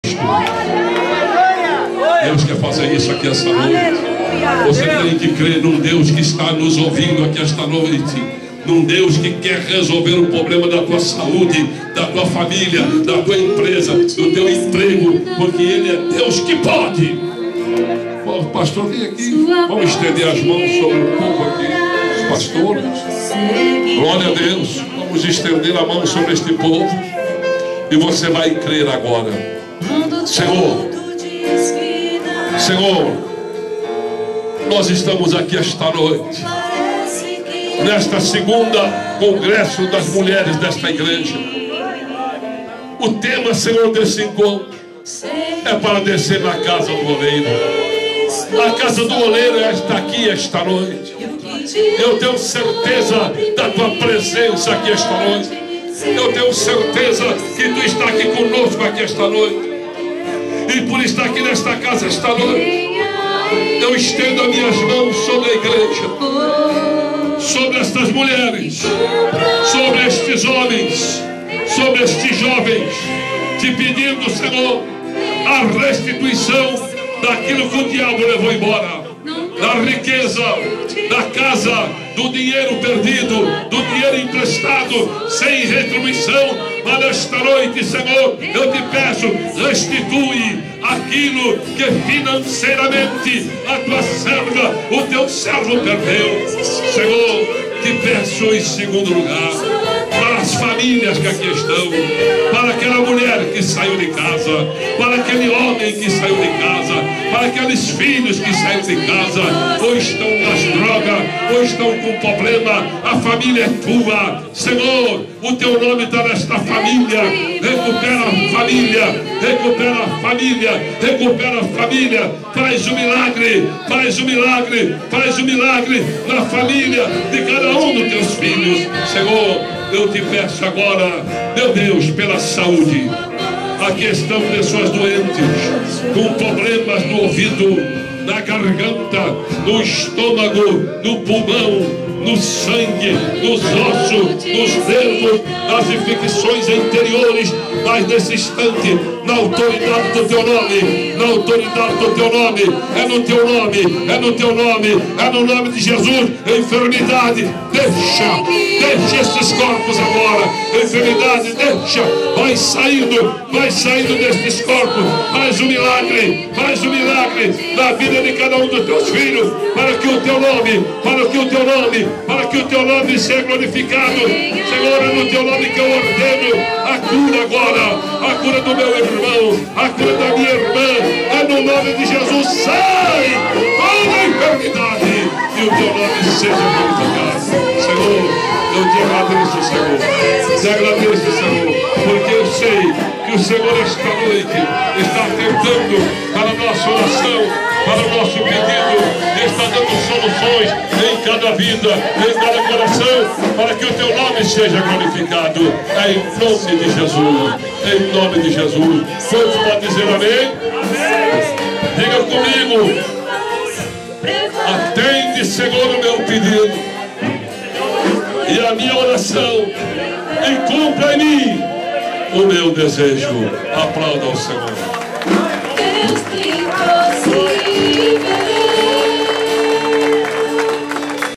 Ministerio da Graça - Pastor Paulo Lucas Sacramento - Parte 4
Pastor Paulo Lucas Sacramento Ministrando no congresso de Mulheres no Ministerio da Graca